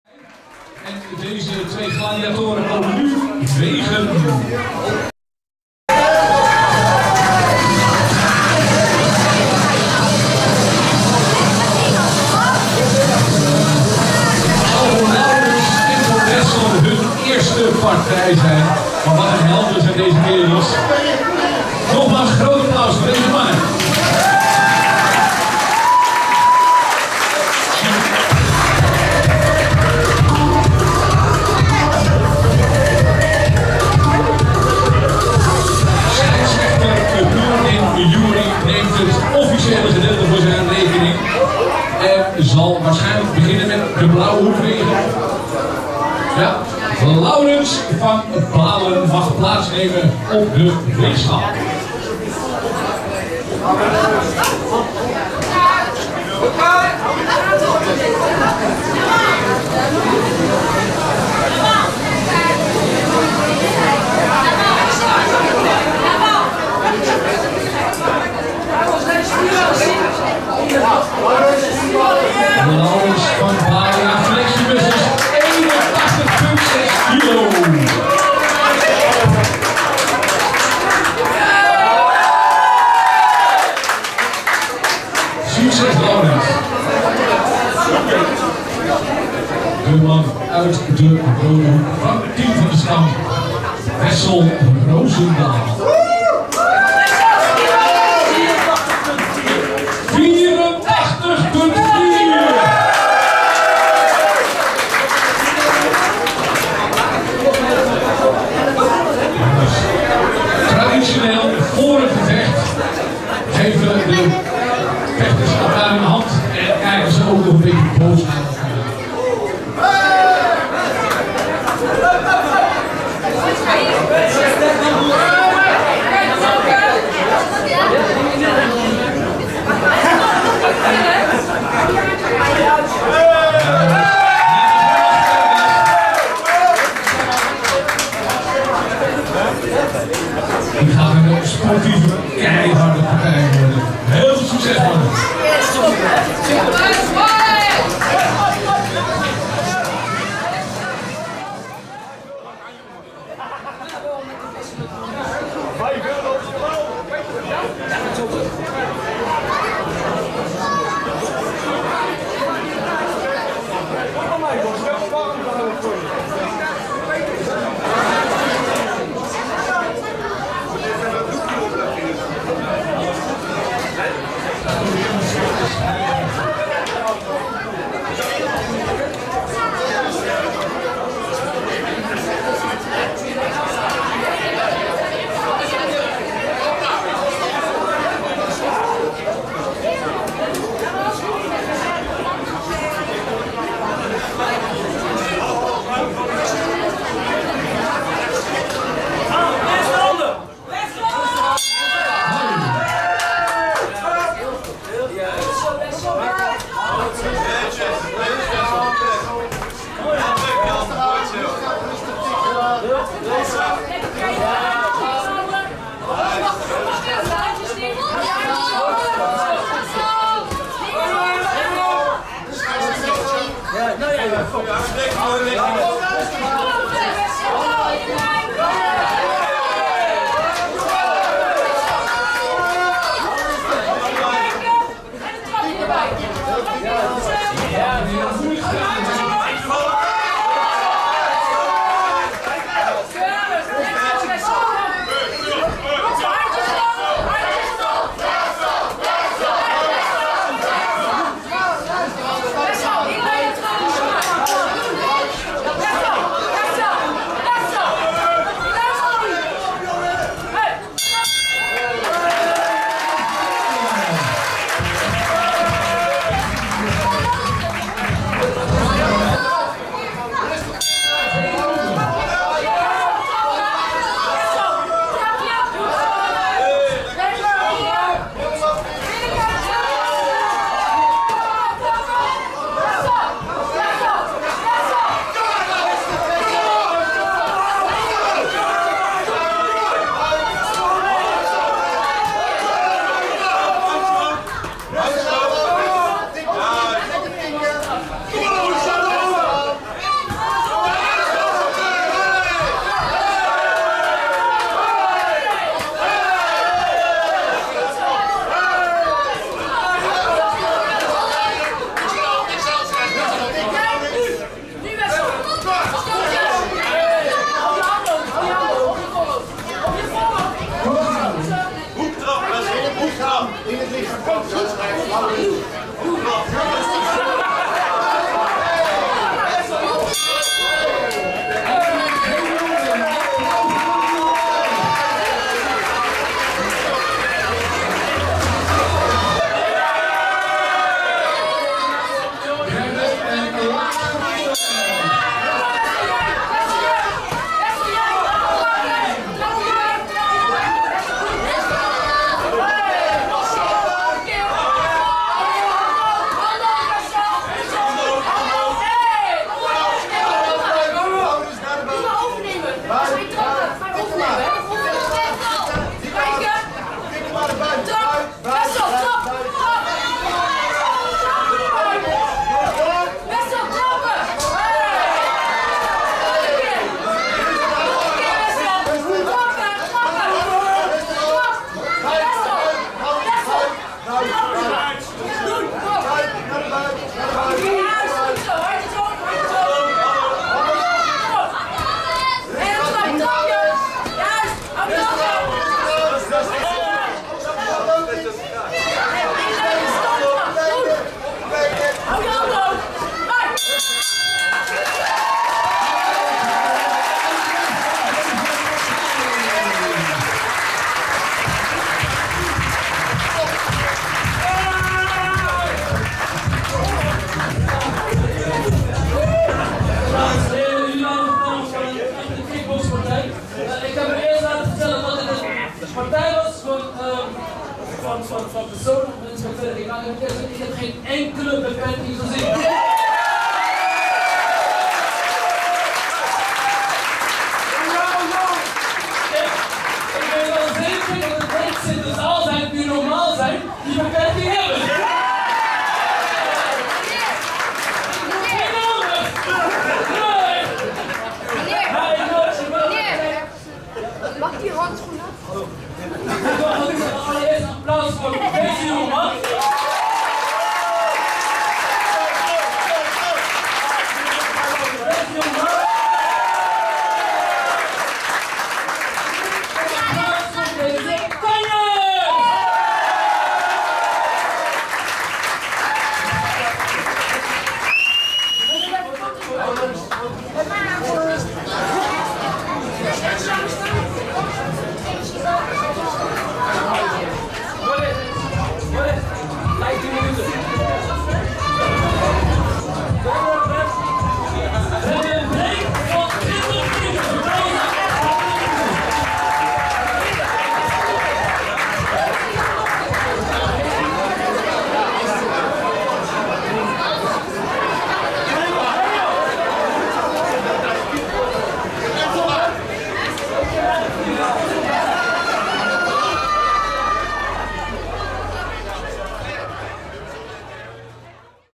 GLOBAL FIGHTS II - Wessel Rozendaal vs Laurens van Balen
Super Special Fight 3x1 minuut
Global Fights Holland part II